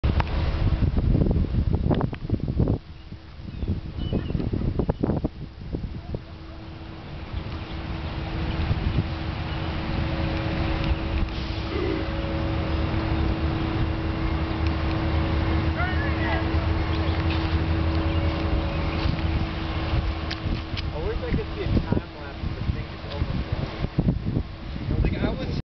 Flooding in Racine Island Park Two
Flooding in Racine Wisconsin at Island Park Part 2.